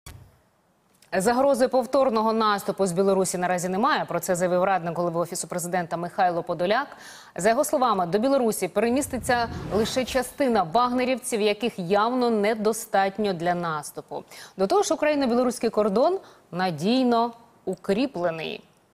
Подоляк оцінив, чи є загроза для України після перекидання "вагнерівців" у Білорусь
Небезпеки наступу з півночі для України зараз немає. Бойовики ПВК "Вагнера", яких перекинули до Білорусі, не становлять загрози.